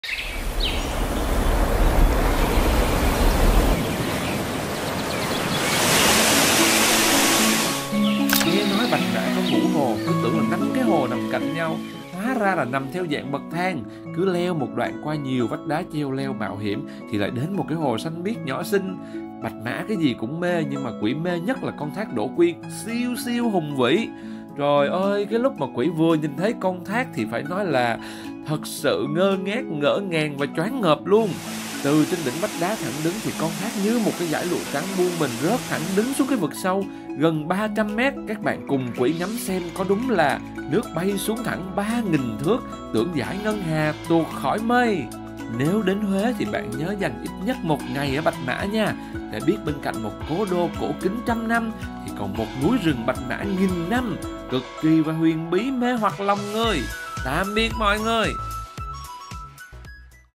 NGỠ NGÀNG THÁC ĐỖ UYÊN | VƯỜN QUỐC GIA BẠCH MÃ
Nước bay xuống thẳng ba nghìn thước
Tưởng dãi ngân hàng tuột khỏi mây

Thác Đỗ Quyên Là một trong những điểm đẹp nổi tiếng, nằm trong Vườn quốc gia Bạch Mã.
Chiều cao của thác Đỗ Quyên là 30m dòng nước chảy kèm theo tiếng nước chảy cuồn cuộn, tiếng lá cây xào xạc và tiếng chim hót véo von tạo nên bản giao hưởng thiên nhiên độc đáo.